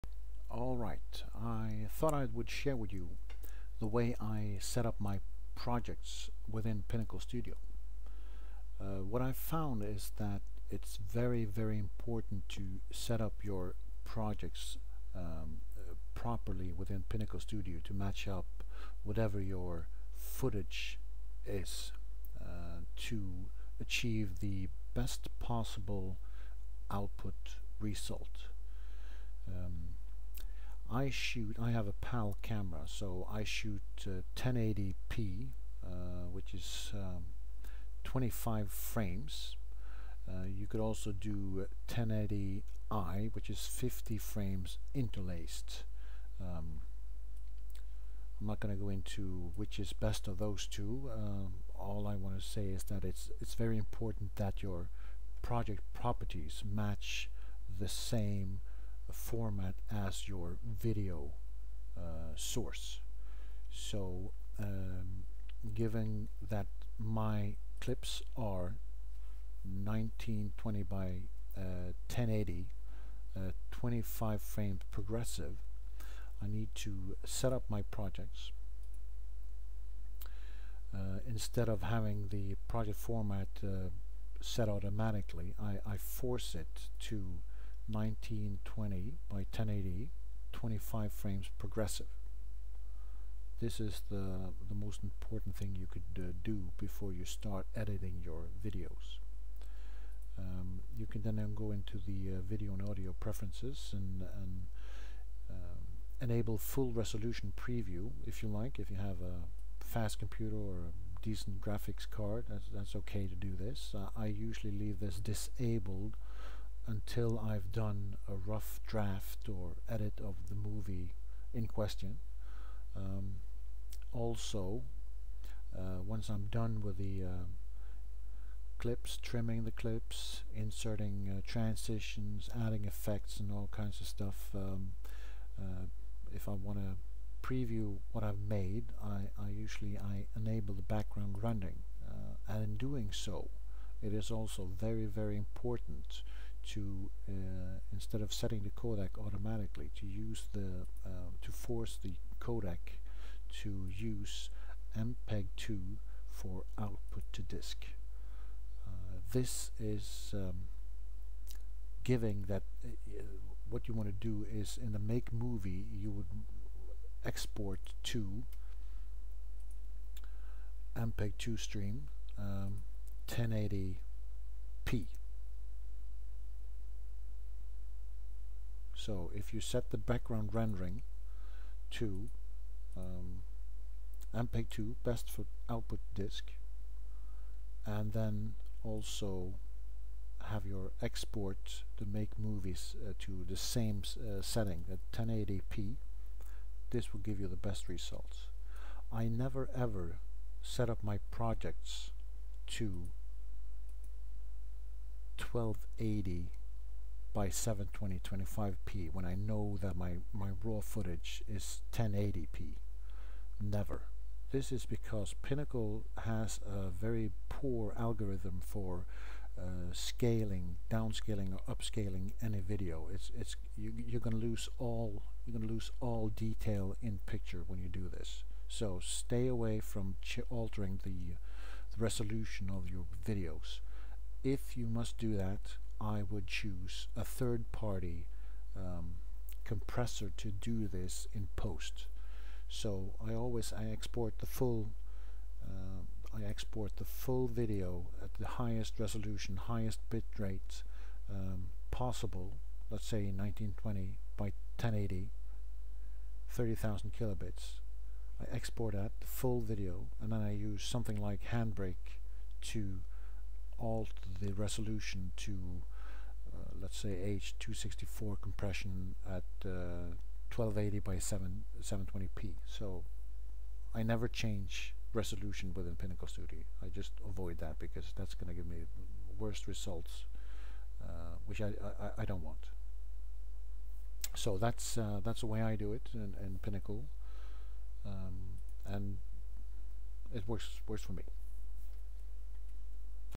How to properly setup Pinnacle Studio project properties
This is how I'm setting up all my Pinnacle Studio projects.